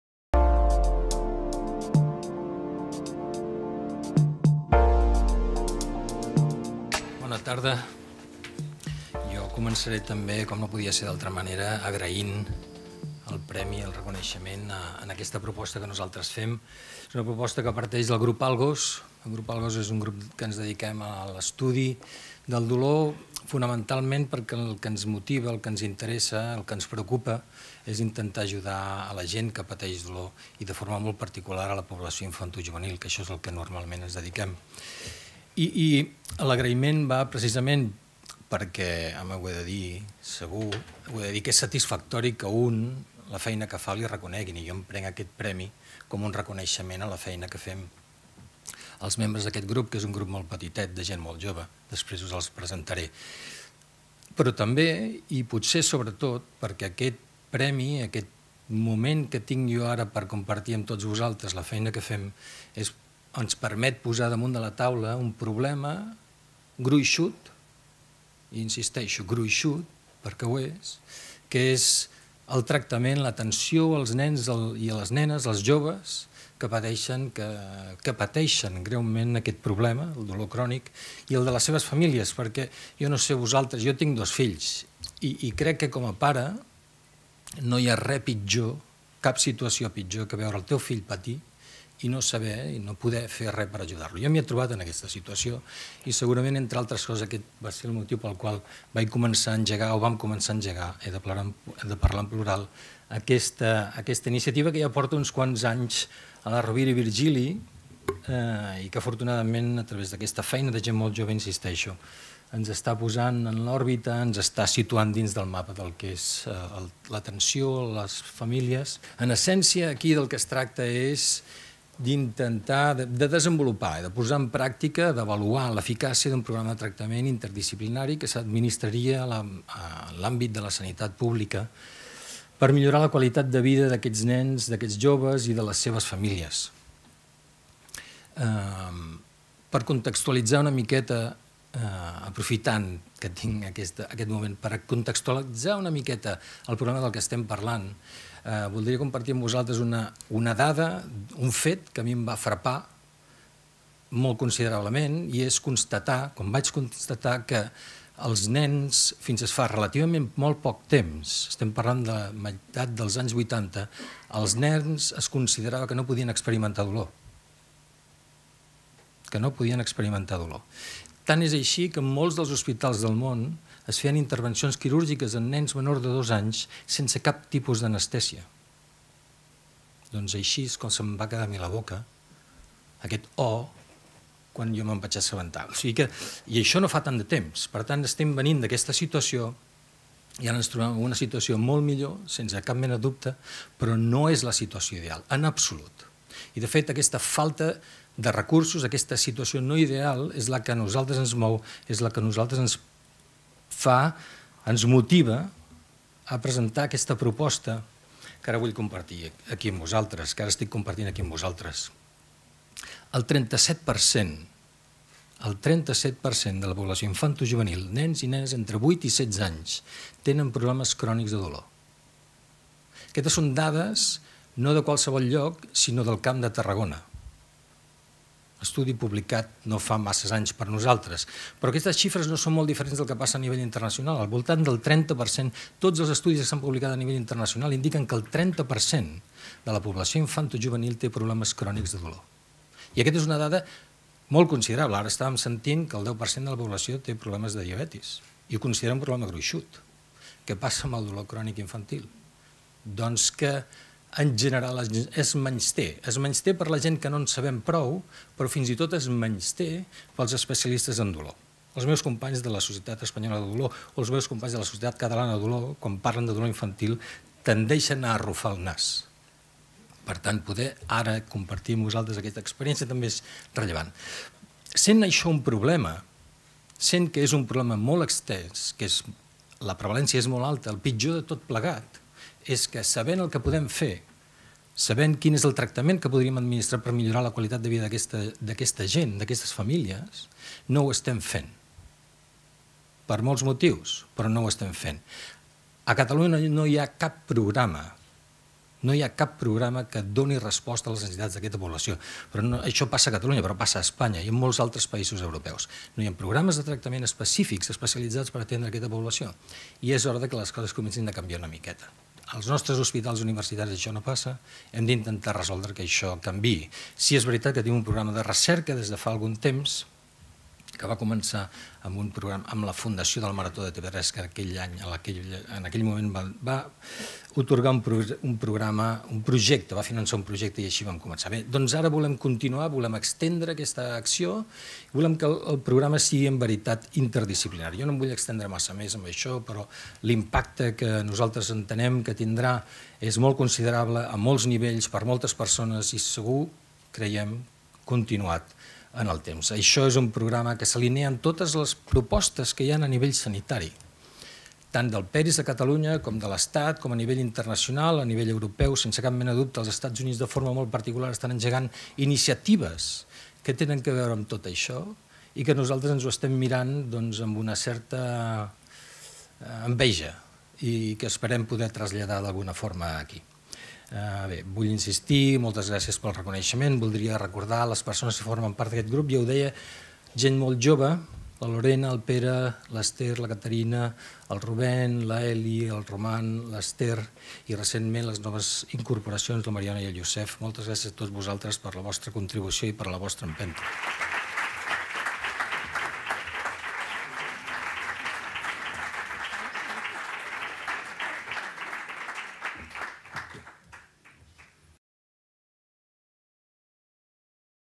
Premi a l'Impacte Social de la Recerca 2018 al grup de recerca ALGOS
El Jurat del Premi Consell Social URV a l’Impacte Social de la Recerca, un cop valorades les propostes d’acord amb els criteris establerts a les bases reguladores del premi, ha decidit atorgar el Premi Consell Social URV a l’Impacte Social de la Recerca de l’àmbit CIÈNCIES DE LA SALUT I DE LA VIDA en la modalitat EX ANTE al projecte titulat: “Tractament interdisciplinari del dolor crònic infantil”, del Grup de Recerca ALGOS. Recerca en dolor, per l’impacte social que pot tenir la implementació i la avaluació d’un programa interdisciplinari per el tractament del dolor crònic infanto juvenil en el context de la Sanitat Pública, amb la finalitat de millorar la qualitat de vida d’aquesta població afectada i la de les seves famílies.